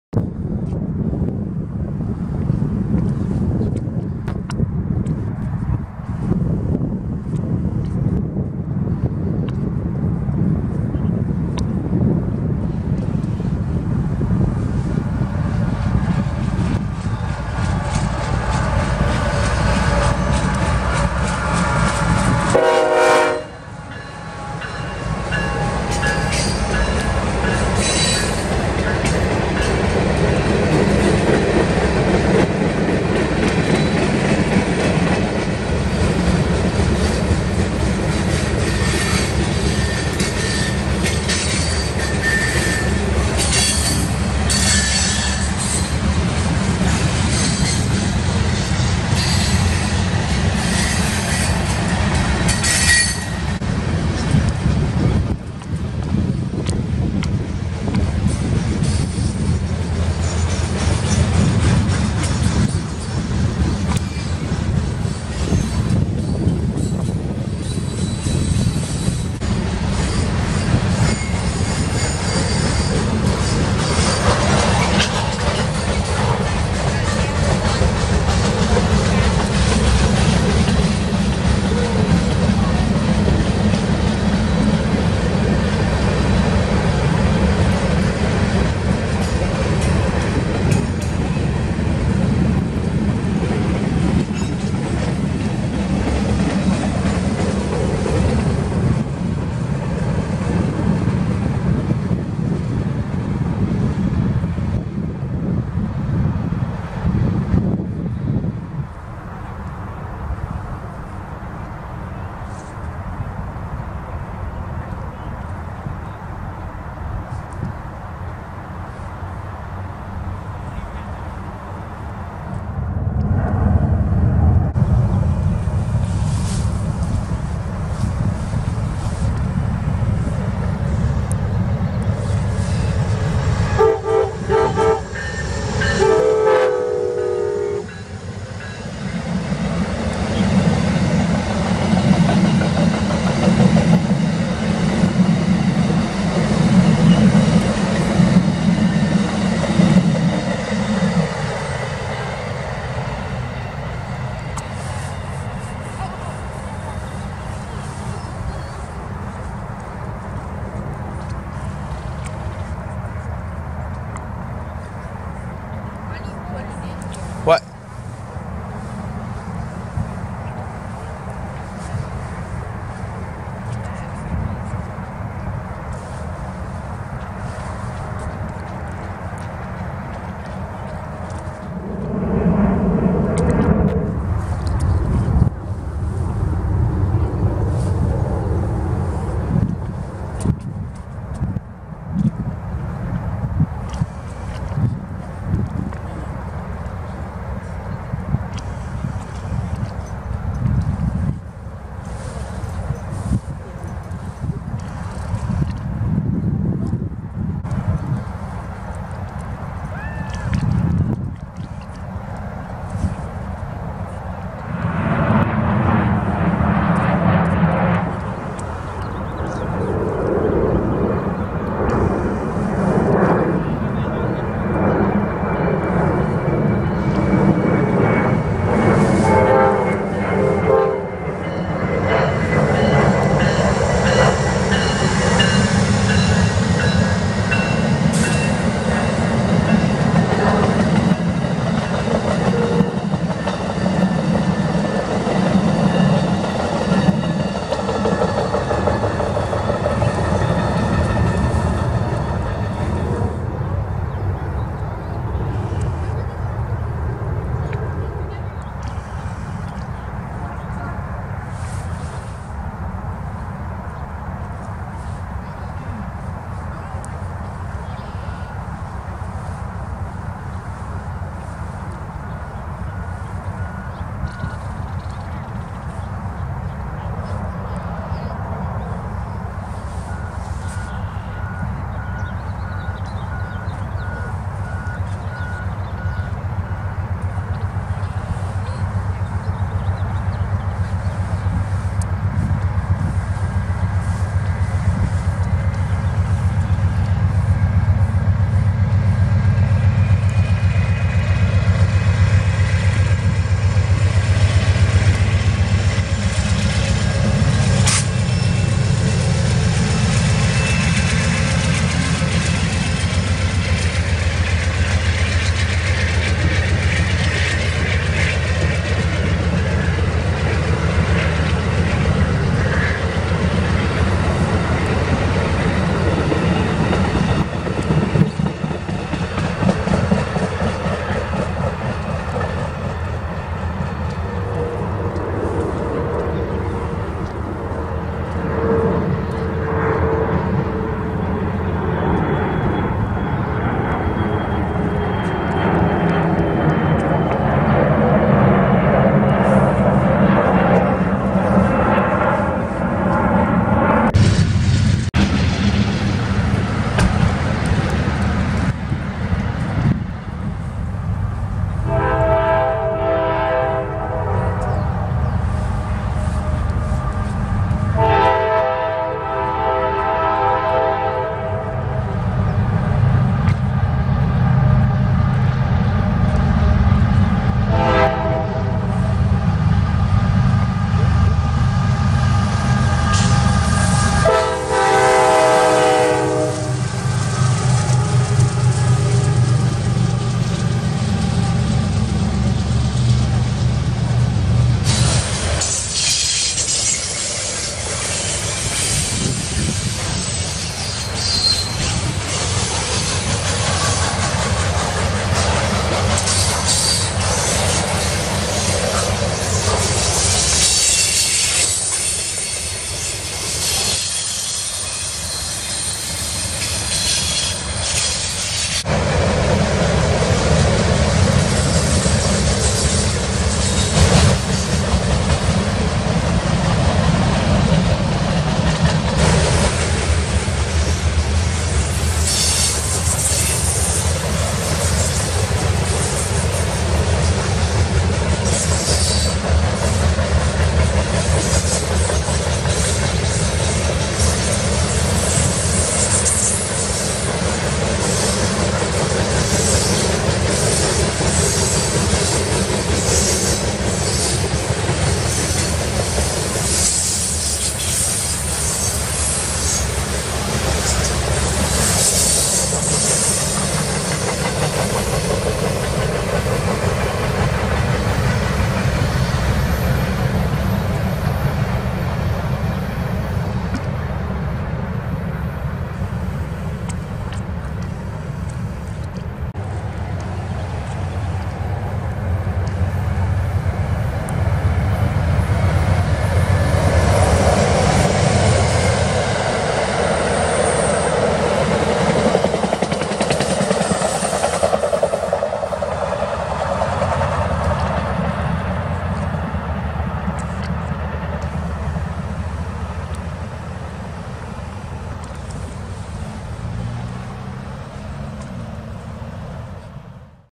4K: Great Day On The CSX RF&P Sub Railfanning with train meets 2/18/18
For the 100 subscriber special I decided to take a trip down to the RF&P and see some Amtrak and also freight